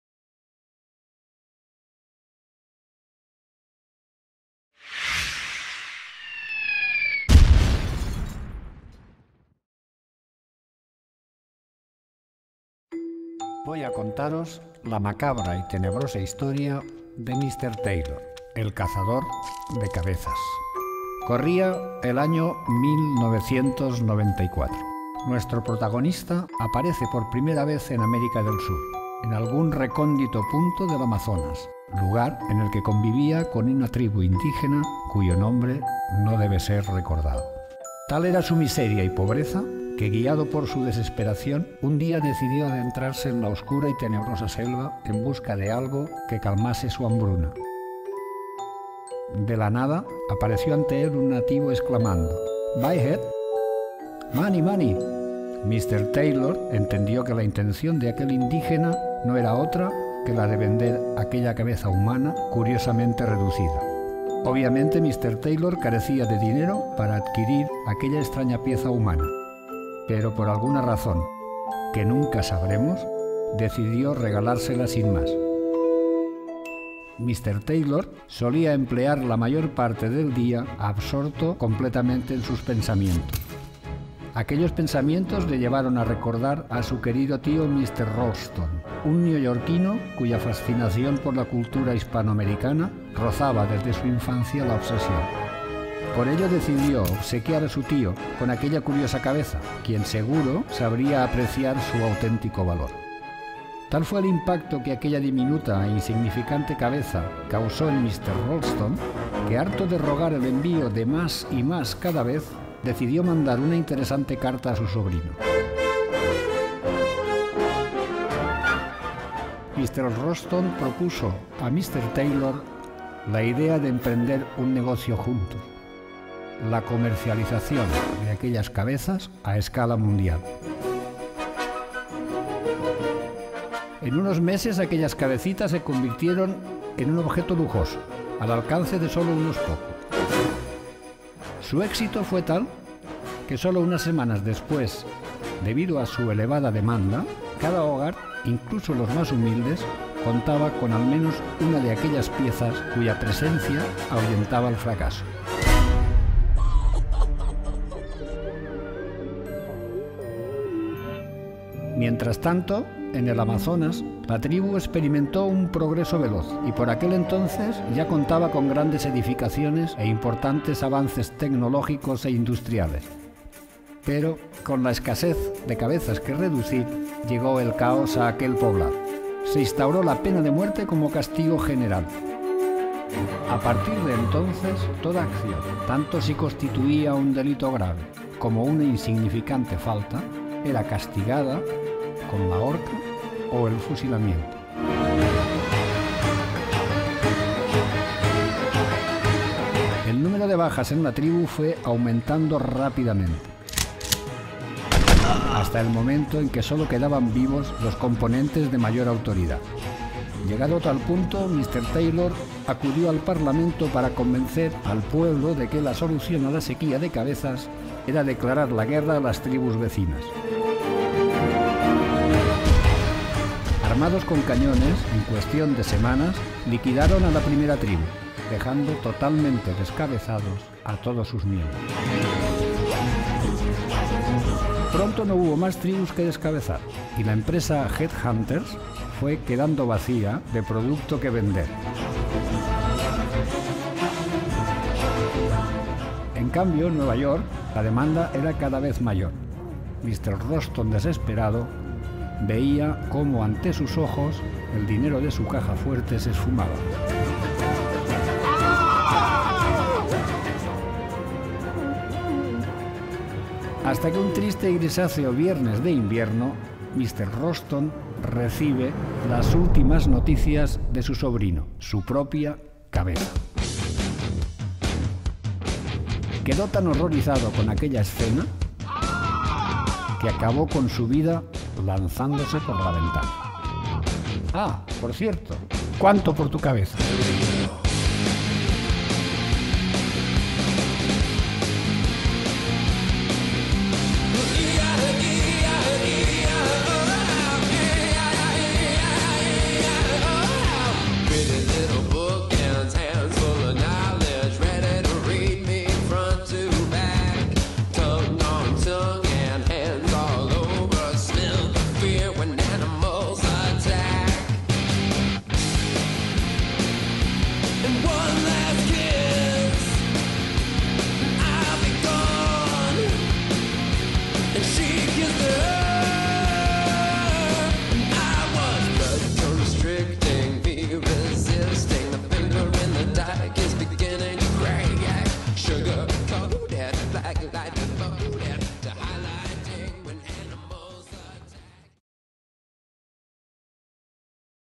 Mr Taylor - Animación del alumno Guillem Zaballos. Diseño Gráfico.
Guillem Zaballos:
"Este es mi primer proyecto para la asignatura de Animación 2d en ESAT (Valencia-España). Se trata de un cortometraje inspirado en la obra Mr.Taylor, escrita por Monterroso. Me inspire en el estilo gráfico de Tim Burton para plasmar el corto. Los audios utilizados son también de películas de Tim Burton."
Trabajo de segundo curso de diseño gráfico.
2º HND in Art & design.